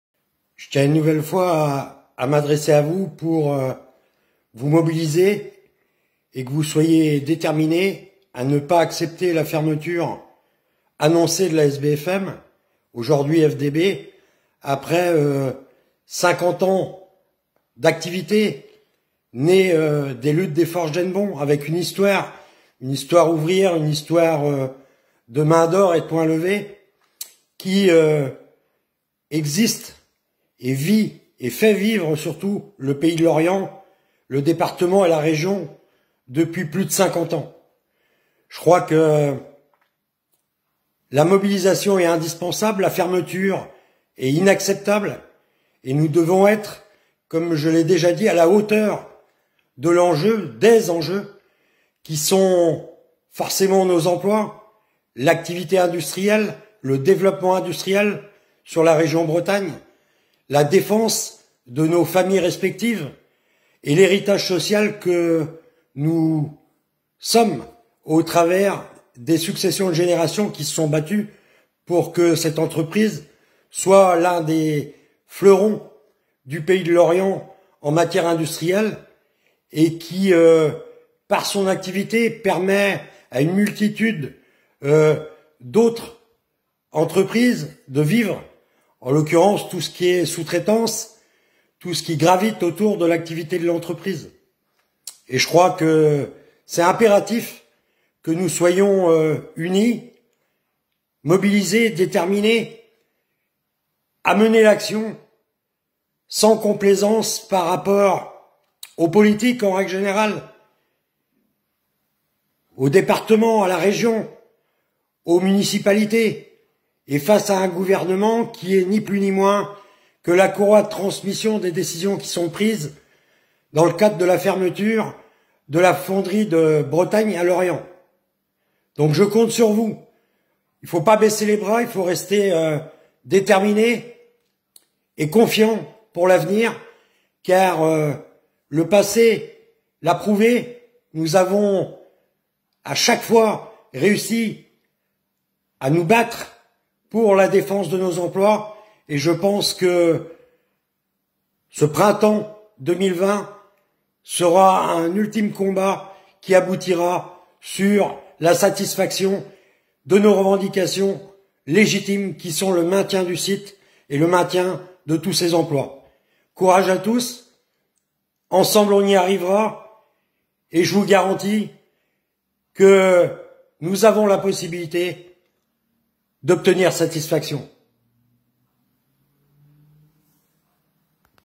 Message de Pierre Le Menahès, ancien secrétaire du syndicat de la SBFM
Message de Pierre Le Menahès, ancien secrétaire du syndicat de la SBFM (aujourd'hui Fonderies de Bretagne), contre la fermture de l’usine : « C'est une histoire de mains d’or et de poings levés, qui vit et fait vivre le pays de Lorient depuis plus de 50 ans. La fermeture est innacceptable. Nous avons à chaque fois réussi à défendre nos emplois. Ce printemps doit être celui de la satisfaction de nos revendications pour le maintien de l’usine et de nos emplois !»